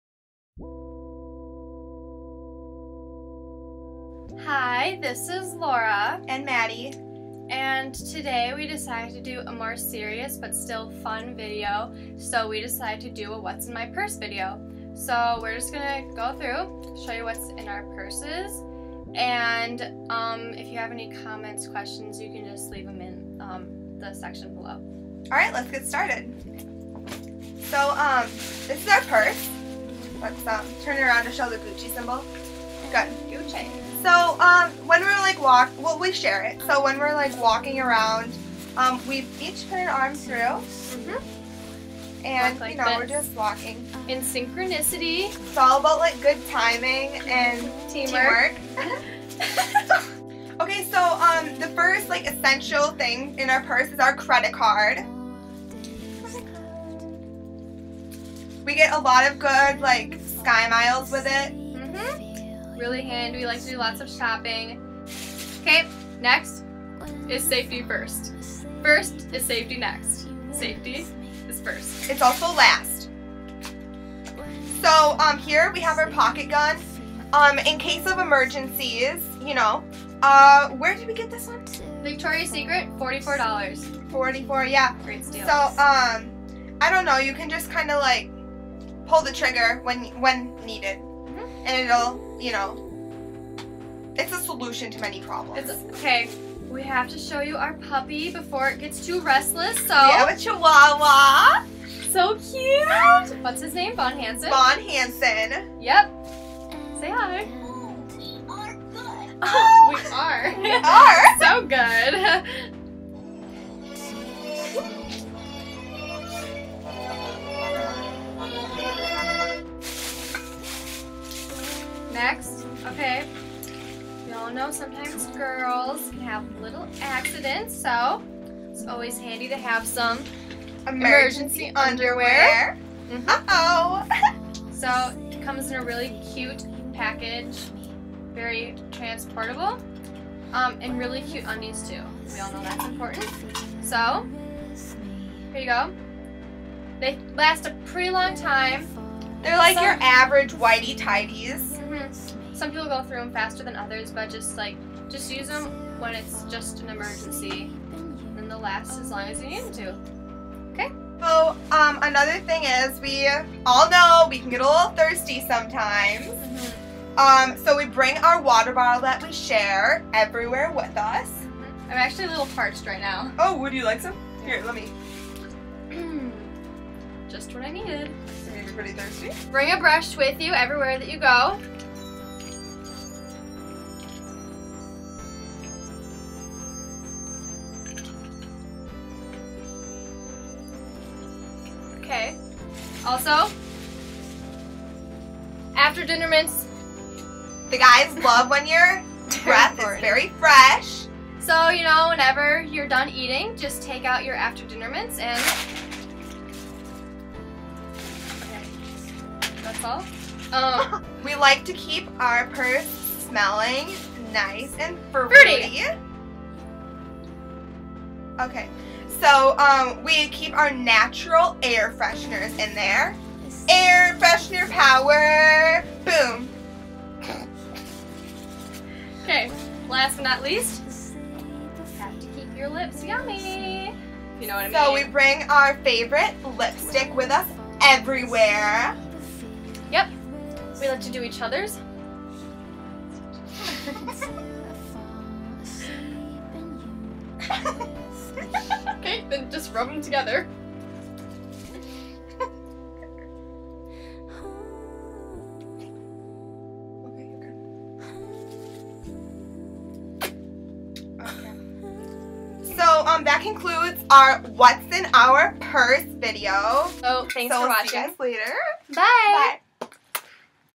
what's in my purse?!
just a little video describing our essential purse items.